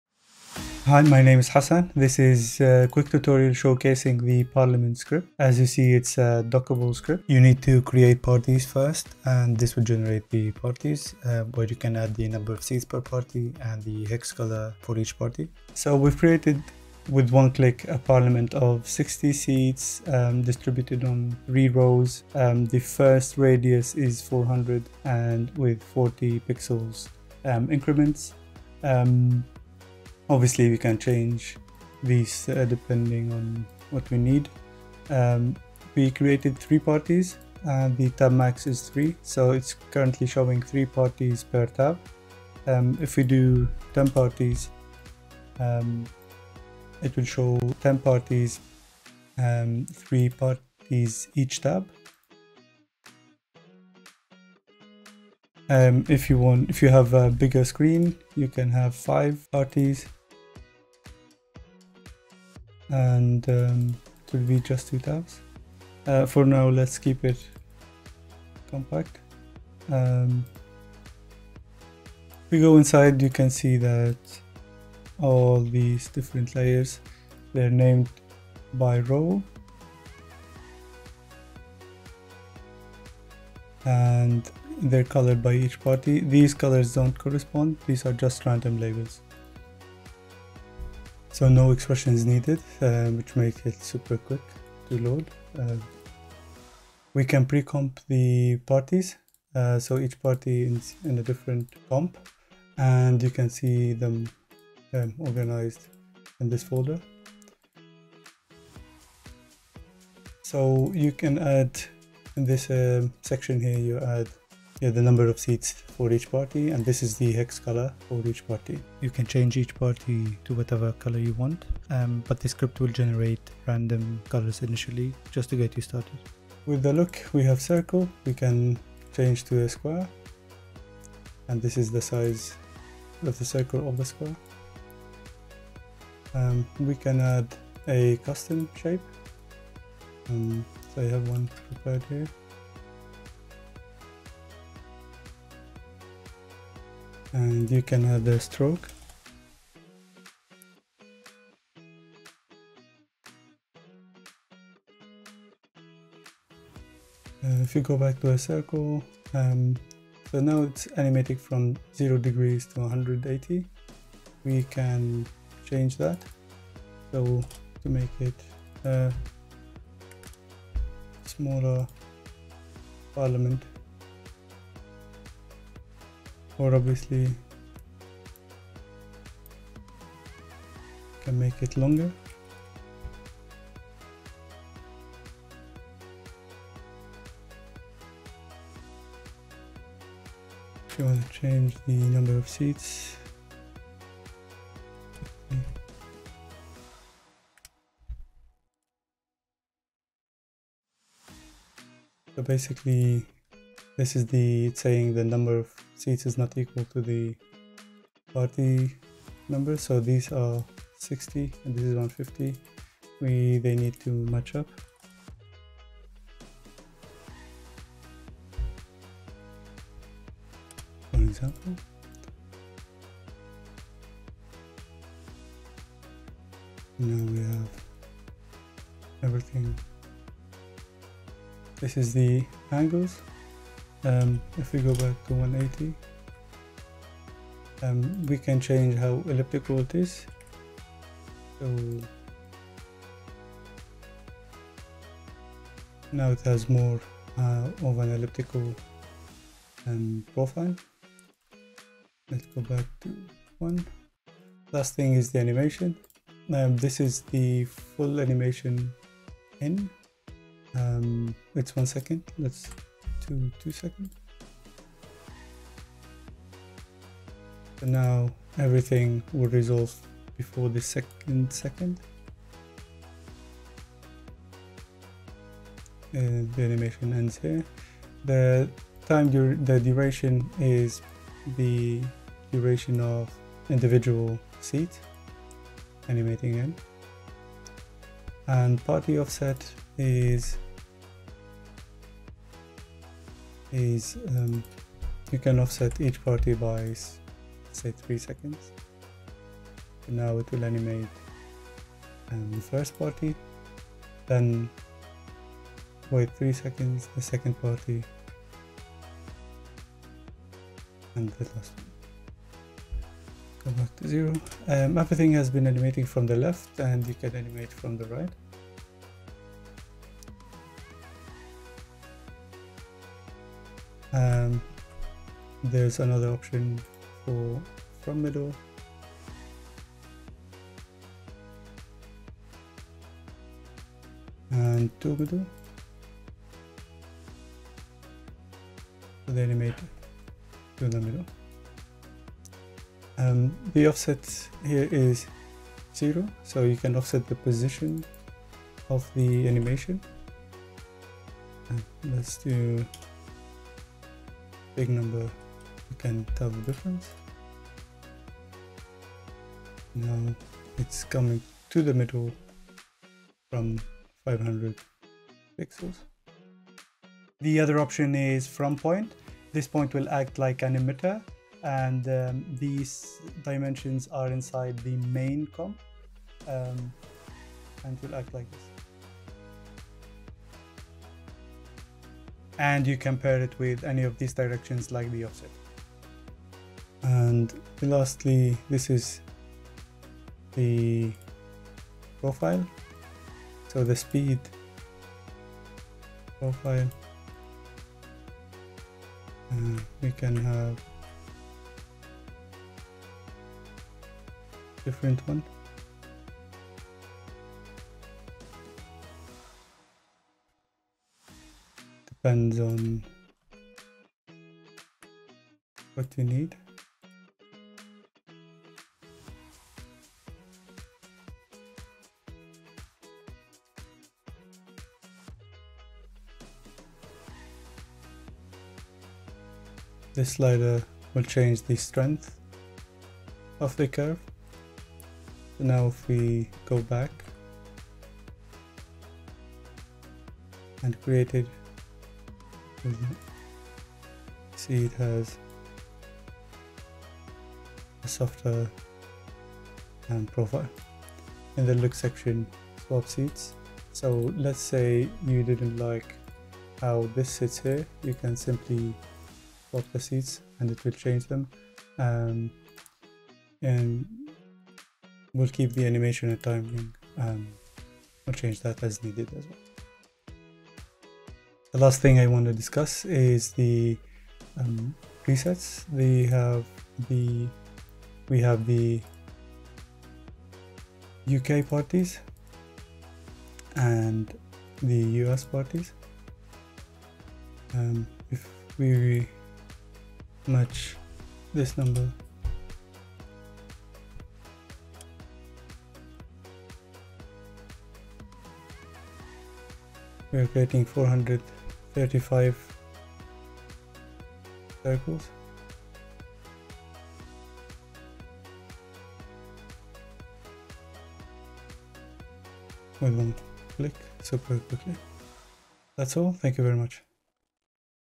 The Parliament for After Effects Tutorial
Create a parliament visual representation with one click !

0:00 UI
0:17 Seats
0:36 Number of Parties
1:36 Pre-comped Setup
2:05 Look / Seat Style
2:17 Custom Seat Style
2:27 Look / Stroke
2:40 Angles
4:16 Animation Durations
5:15 Animation Directions
5:43 Positional Offsets
6:27 Speeds
7:31 Swapping Seats
7:50 Party Presets